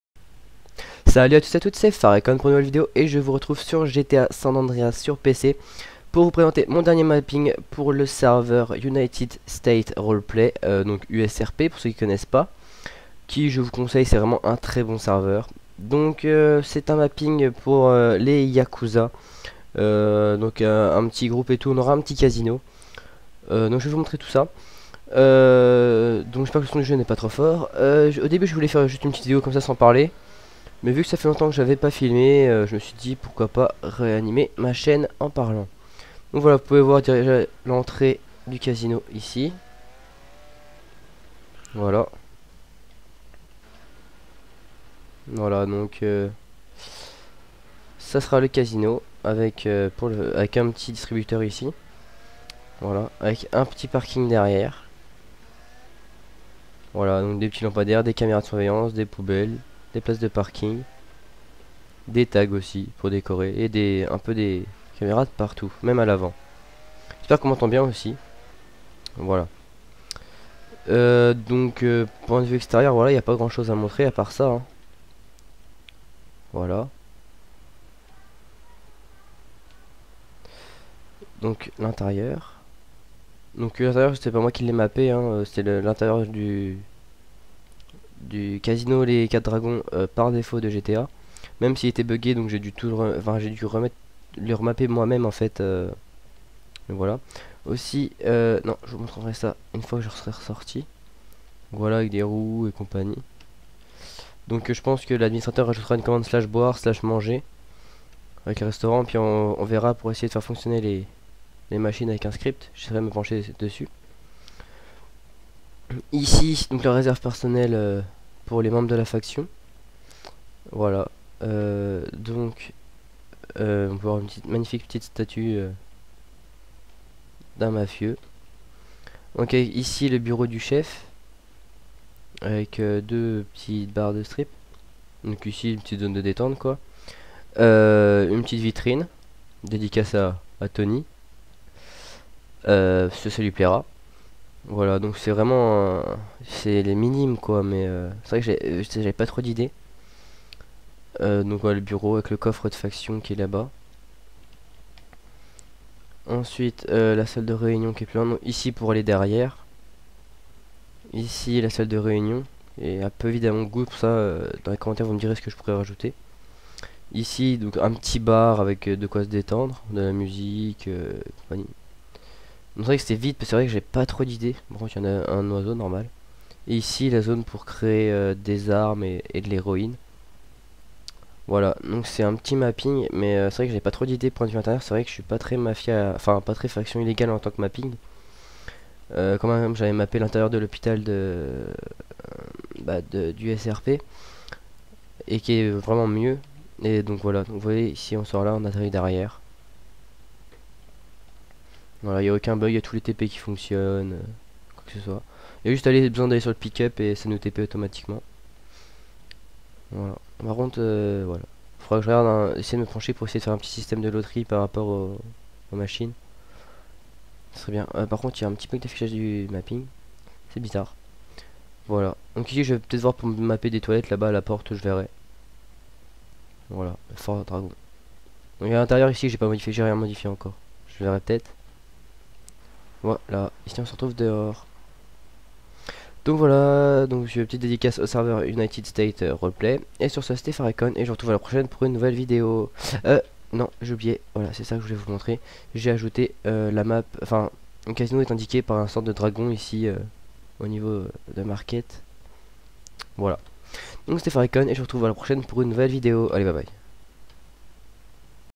USRP - Mapping Casino/Yakuza
Salut tout le monde, voici une proposition de mapping ( a la demande des admins ) pour le serveur USRP ) pour notre organisation les Yakuza.

Donner vos avis ci-dessous.